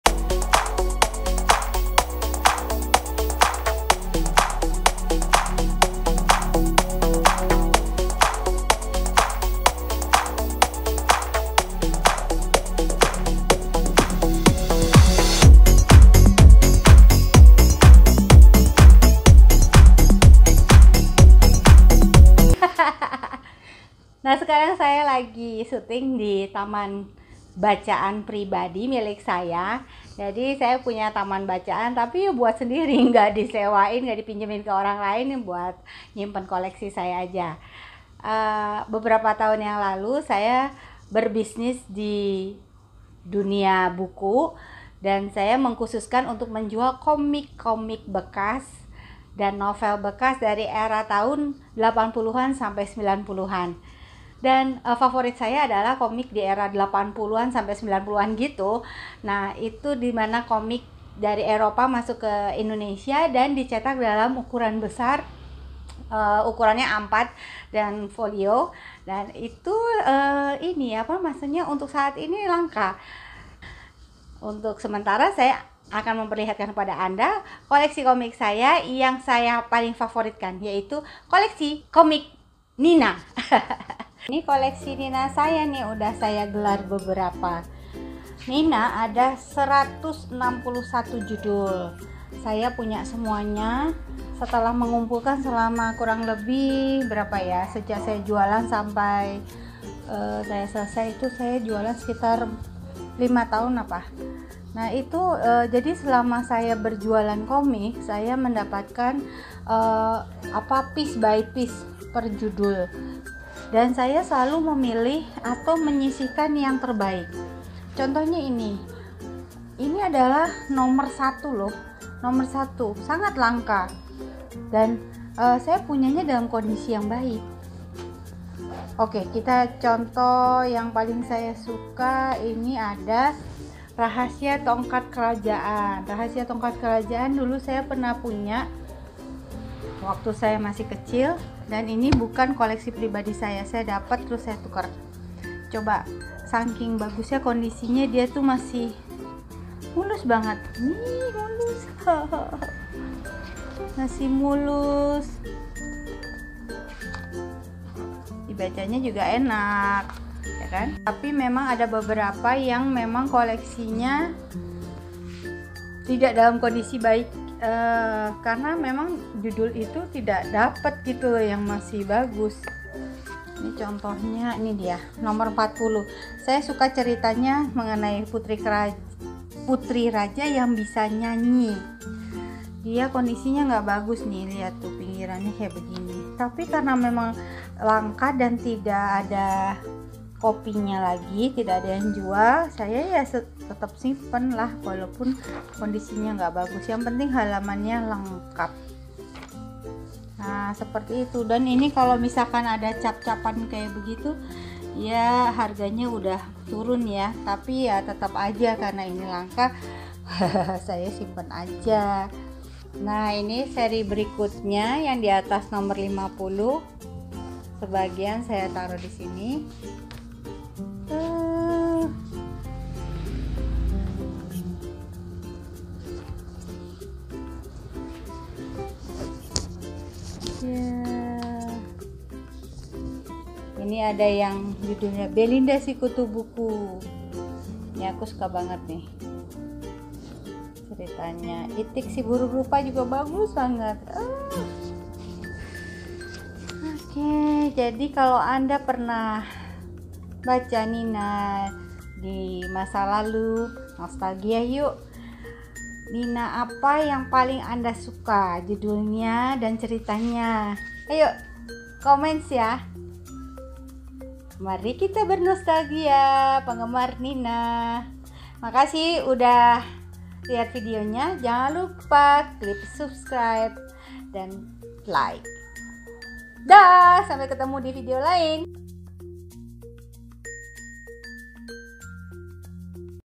Old Rare Comics Collection Tina 1980 series Koleksi Komik Nina Lengkap 161 Judul
Komik 90's koleksi pribadi, ini saya kumpulkan bukan dari usia saya kecil, tapi ketika saya menjadi wira usaha penjual komik bekas. Akhirnya komik yang saya jual, saya simpan sendiri. Saya menjual komik Nina dobelnya.

silahkan yang mau beli produk nina di klik link shop saya